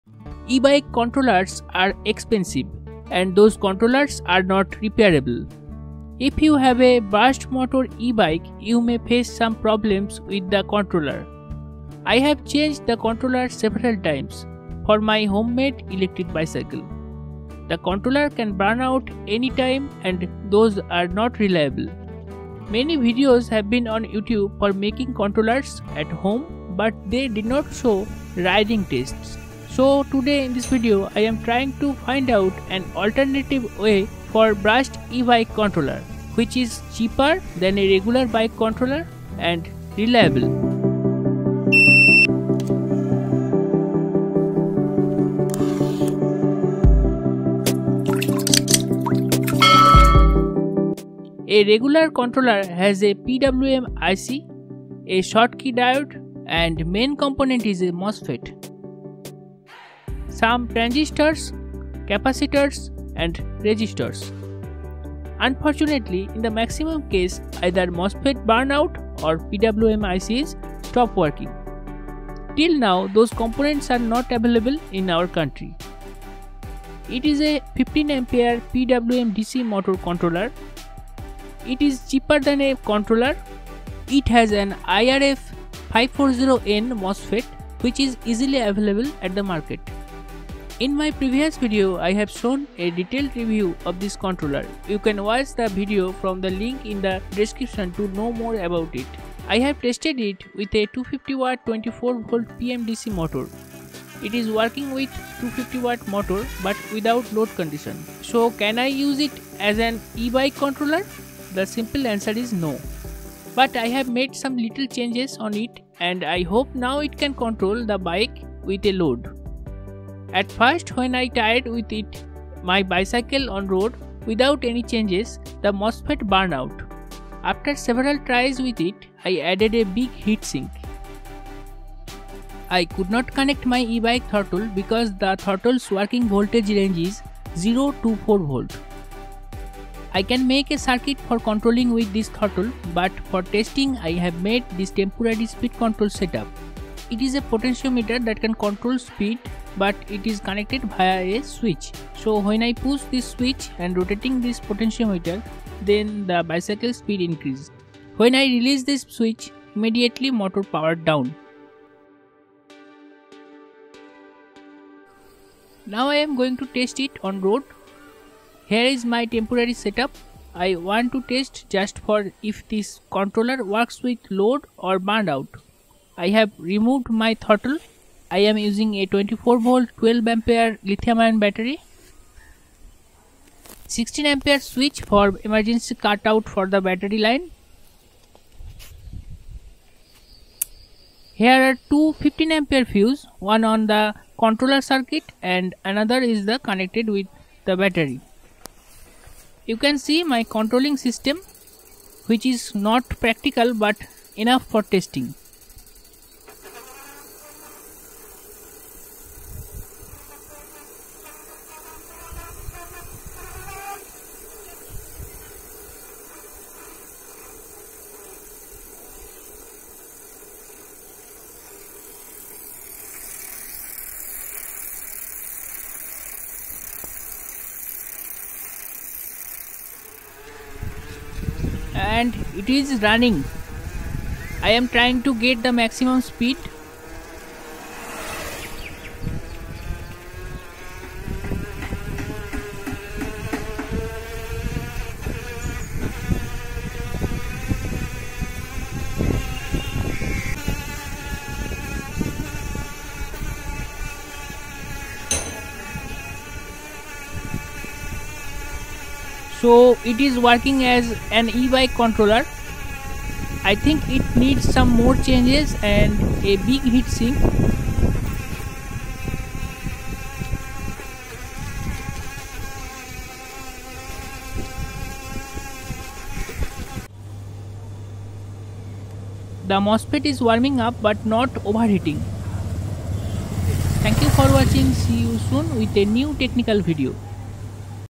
DIY CONTROLLER FOR E-BIKE
An alternative way for 250 Watt 24volt brushed motor controller for electric bicycles. Here I have used a 6V to 90V 15 amp PWM motor controller to control my homemade electric bicycle.
This Ebike controller is cheap & repairable.